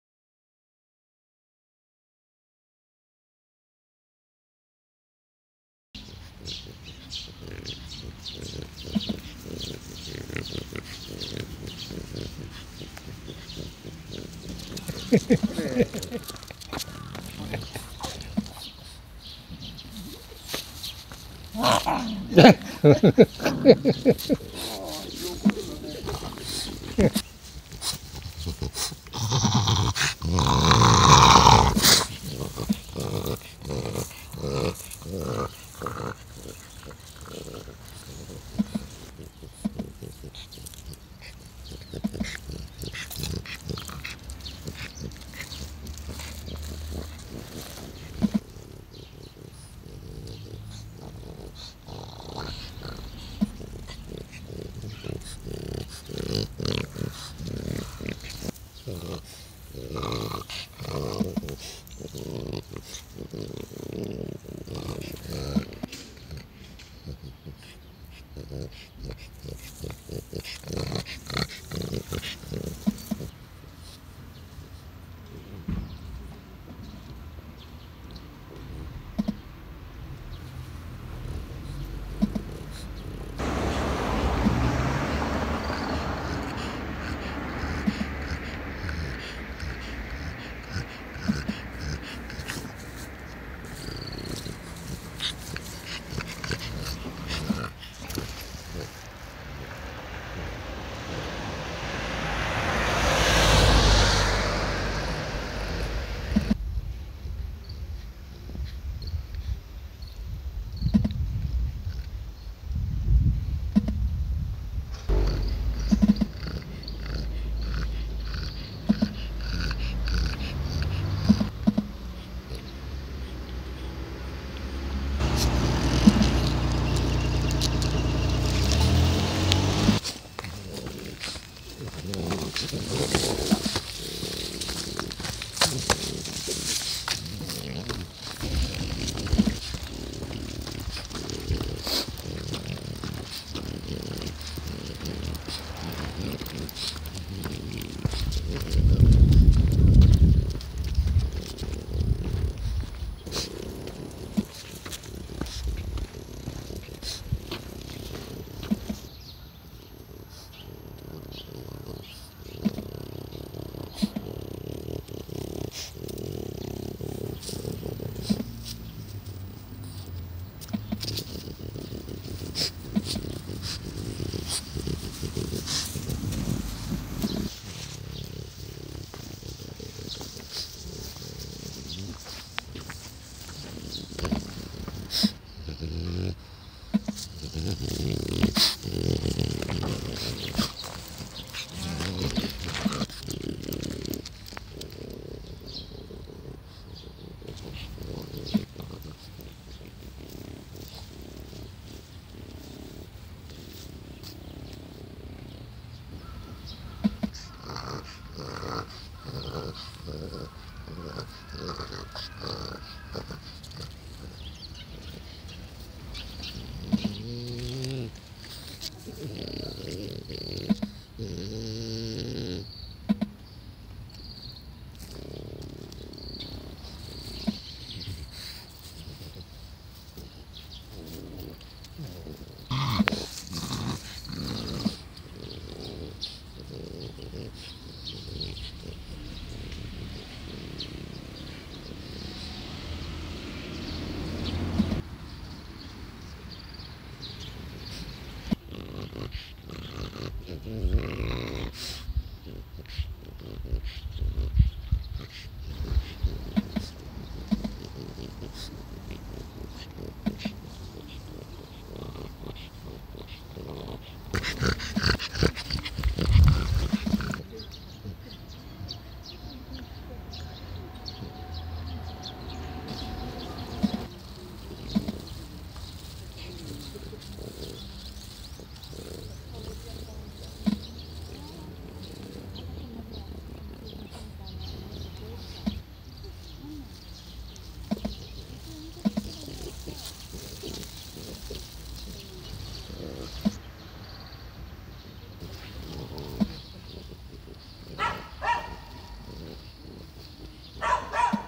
フレンチブルドッグ老犬タロウ（French ｂｕｌｌｄｏｇ）の散歩 ２０１６．０7．15フレブル
フレブルの老犬タロウ12歳です。